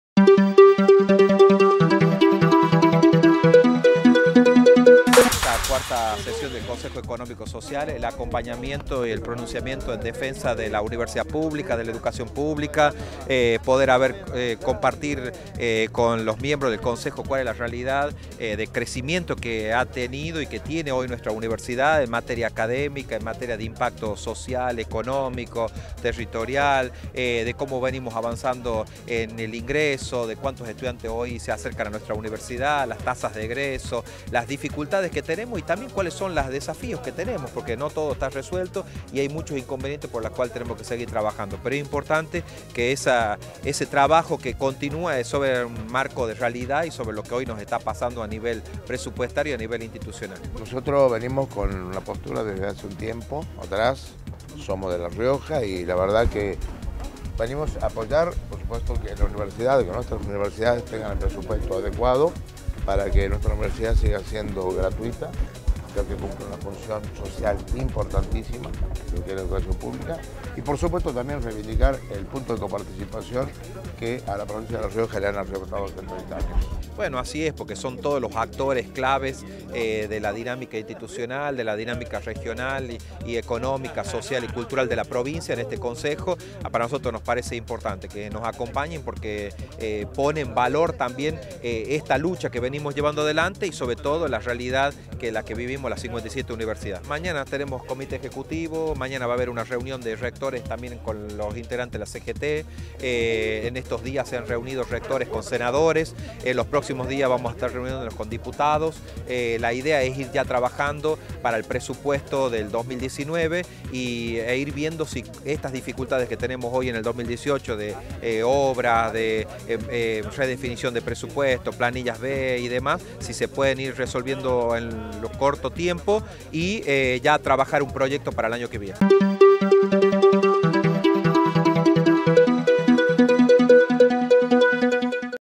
U Noticias - El Consejo Económico y Social sesionó en la UNLaR
Se realizó en la UNLaR la cuarta Asamblea Ordinaria del Consejo Económico y Social en la Sala de la Reforma  del Consejo Superior de la UNLaR; oportunidad en la que los miembros brindaron su apoyo al reclamo por un presupuesto 2019